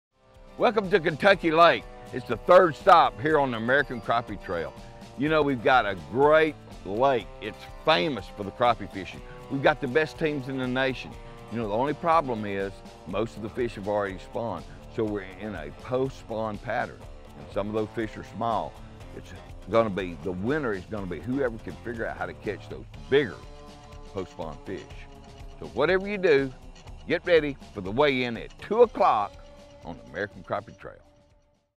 Opening Day on Kentucky Lake
Welcome to the 3rd stop on the American Crappie Trail. The fishing is on!! Who will figure out the key to the big bite to mak ethe top ten going into Championship Saturday. Tune in today for our pregame show with your hosts Sam Heaton and Matt Morgan starting at 2:00pm CST with the live weigh-in to follow at 2:30.